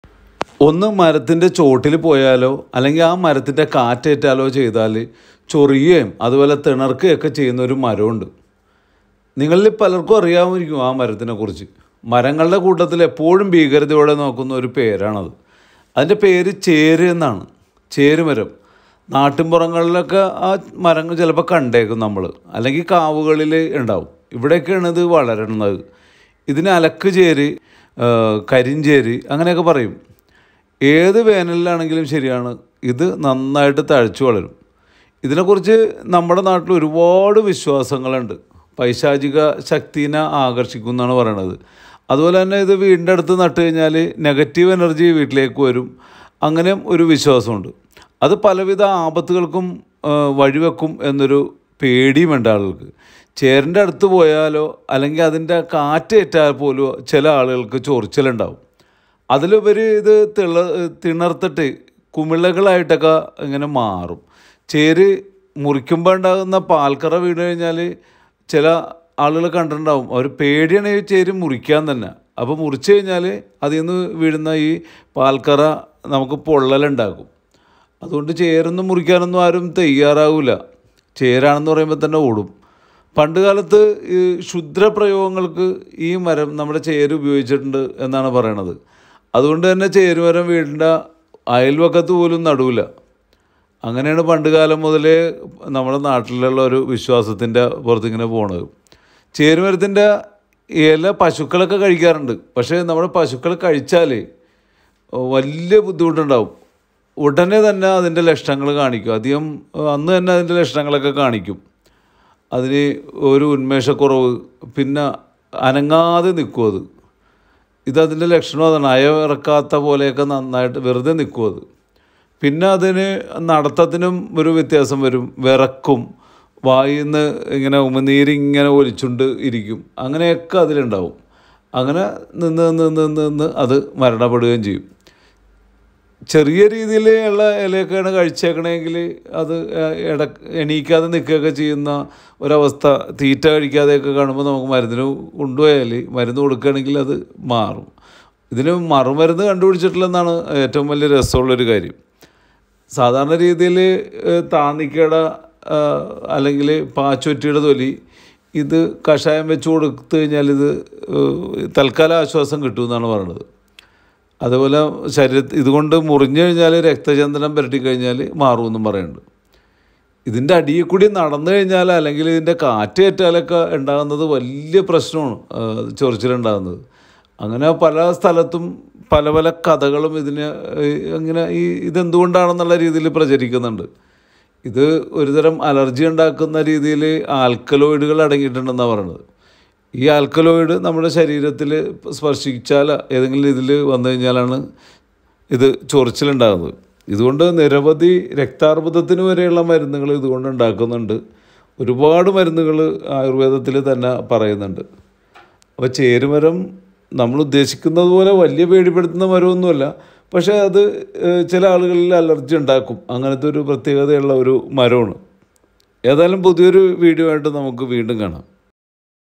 cherumaram
There is a tree that itches and itches if it is stepped on or blown by the wind.
Cheru is a plant that thrives in the countryside.
It has the name Alakucheru.
Holigarna arnottiana
കാട്ടുചേര്, ചേരൽ, ചേറ്മരം, ചേര, കാട്ടുചേരൽ